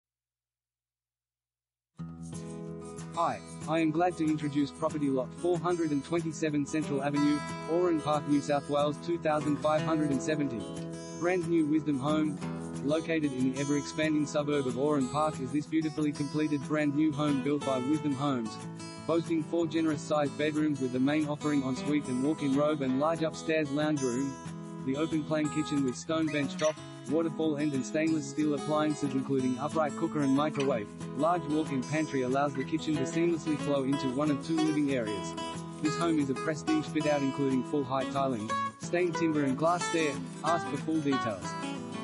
Lot 427 Central Avenue, Oran Park NSW 2570 | AuPropertyReport.Com
Lot 427 Central Avenue, Oran Park NSW 2570
Property Type: house
Bedrooms: 4
Bathrooms: 4
Car Space: 2
Brand New Wisdom Home